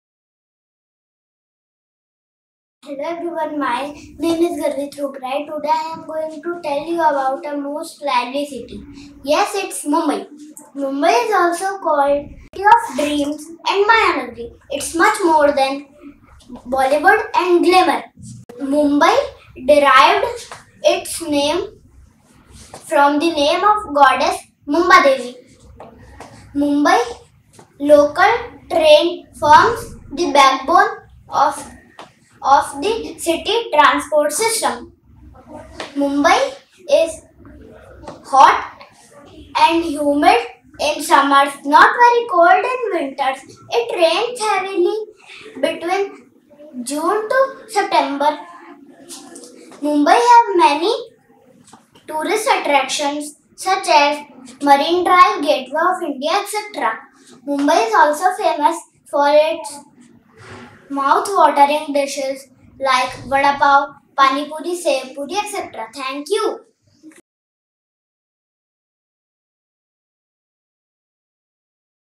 Speaking activity ‘My Favourite City’.
Students of Grade III participated in a speaking activity ‘My Favourite City’. All the students chose their favourite city from West India and shared their thoughts on that city’s culture, languages spoken, famous tourist places, and more. This helped them to learn about different cities of West India and enhance their public speaking skills.